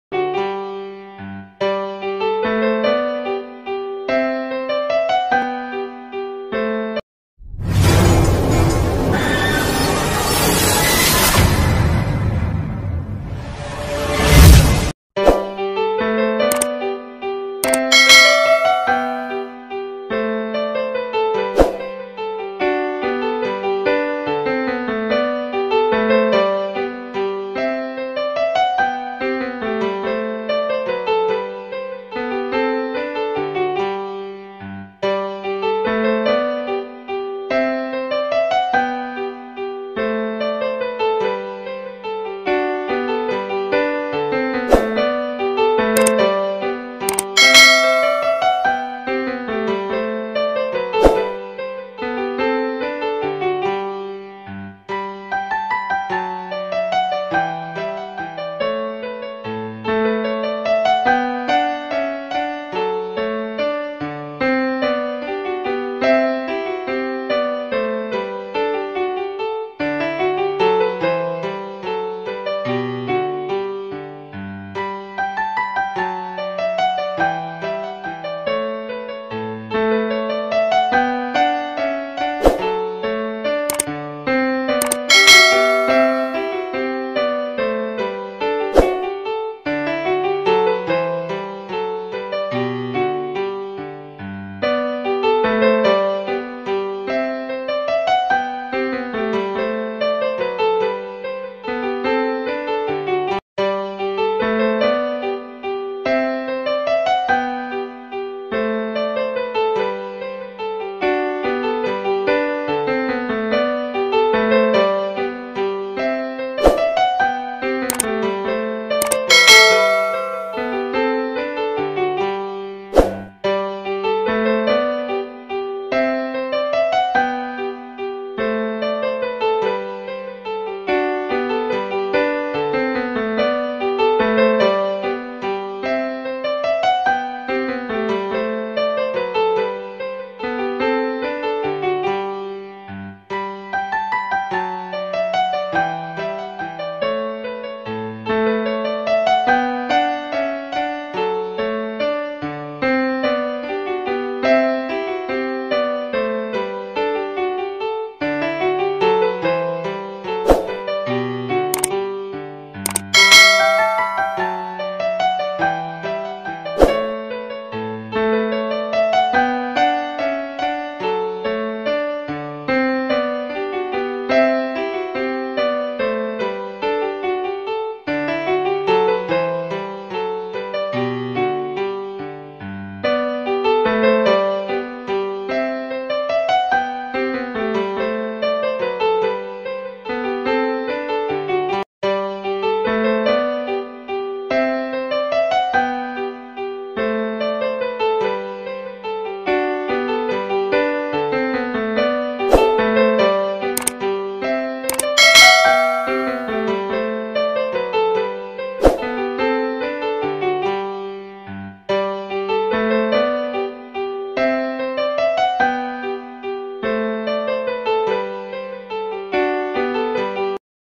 How to paint beautiful scenery with poster colour
Learn How to paint a simple landscape  with Poster colour